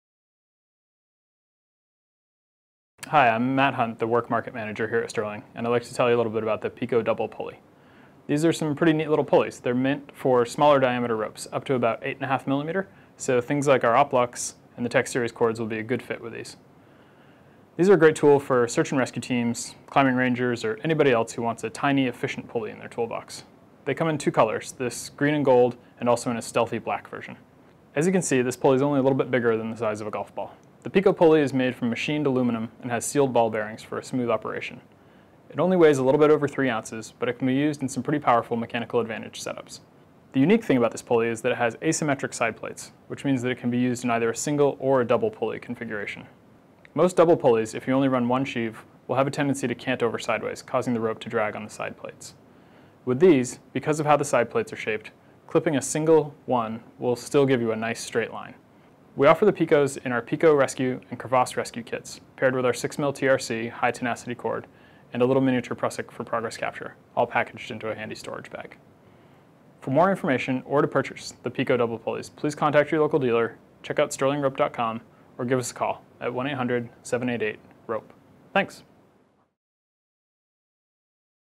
Sterling Pico Double Pulley
Matt Hunt, Work Market Manager at Sterling, talks about the Pico Double Pulley, a compact yet powerful pulley roughly the size of a golf ball. Able to be used for single and double pulley use, due to a balanced side plate.